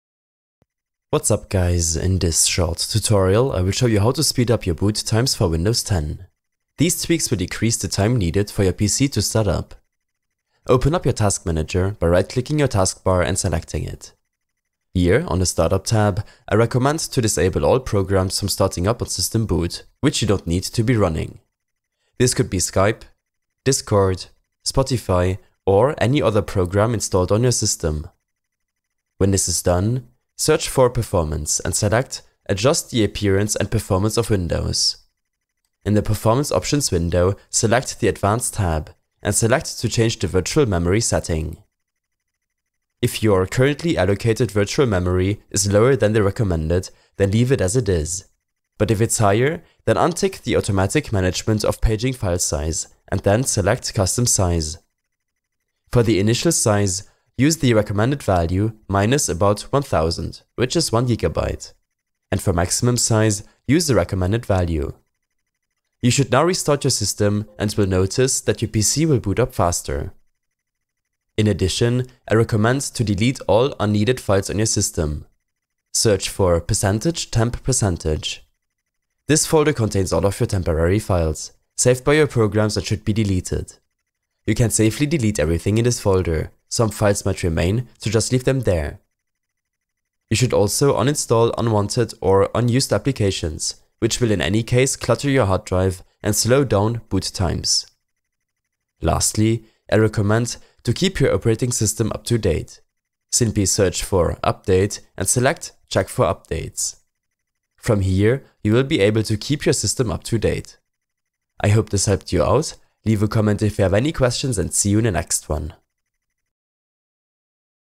Slow Boot Times in Windows 10 - FIX
A short tutorial on how to fix slow boot times in Windows 10.
These tweaks will help you speed up your PC start up time.